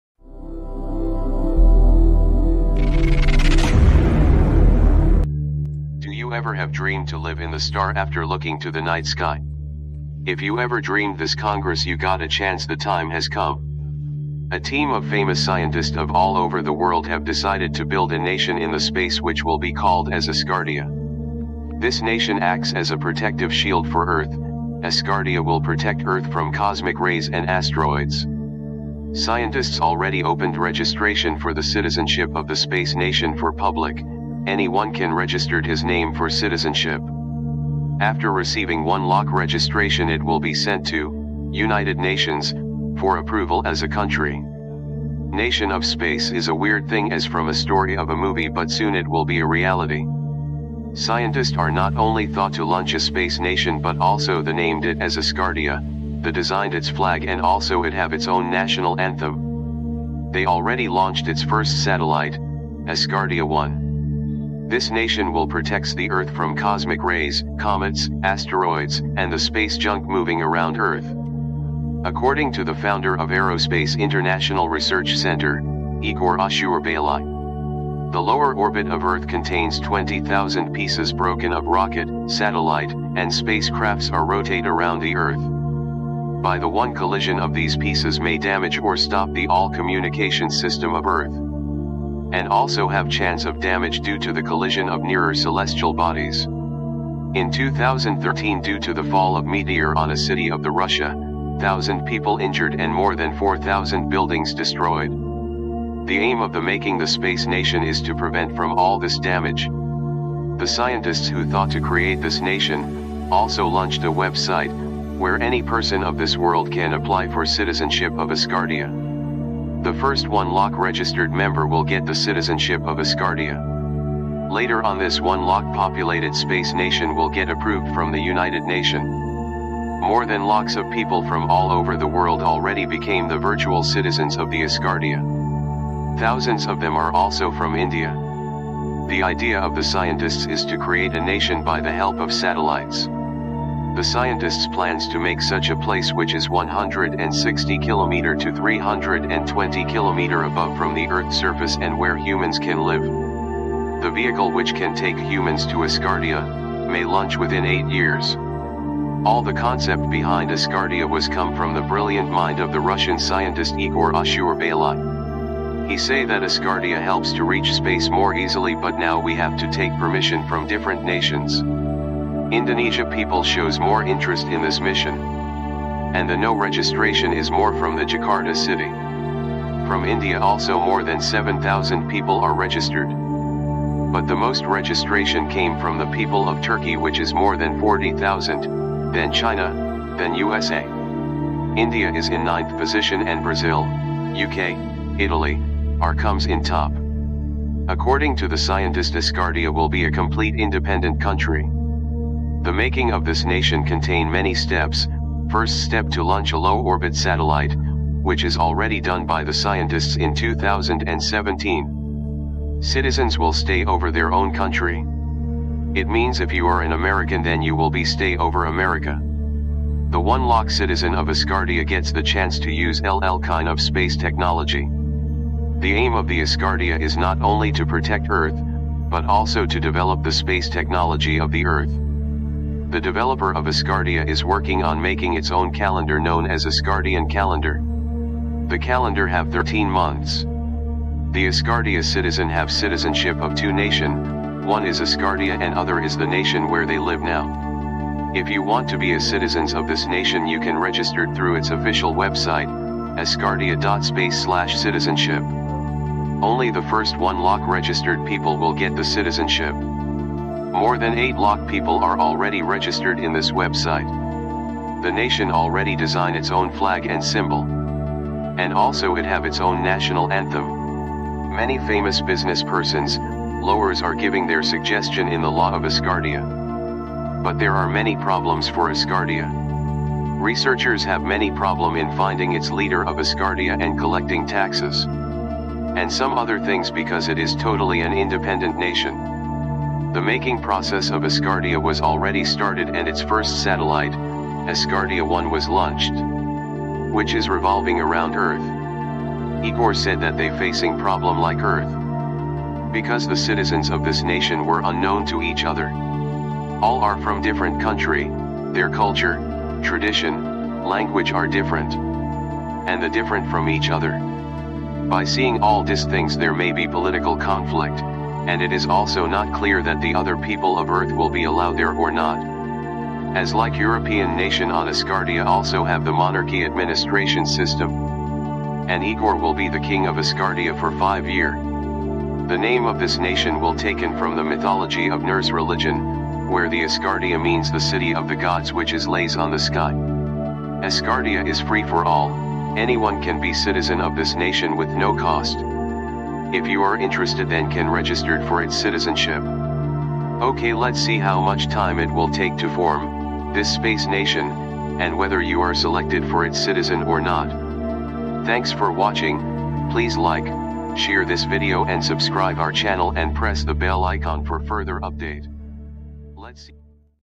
ASGARDIA : THE SPACE NATION 🚀🏞🌌
Asgardia.space/citizenship.   This is the official website for registration.
Asgardia.
Asgardia is the first nation designed and planed by the Igor Ashurbeyli which is the astronaut. This is the nation formed by the satellites. Its construction was already started. In this video you will be see some clips of this space nation . Registration for its citizenship was already started. You can register through the above link.

I hope this video will be give some information about Asgardia.

Amazing World. Asgardia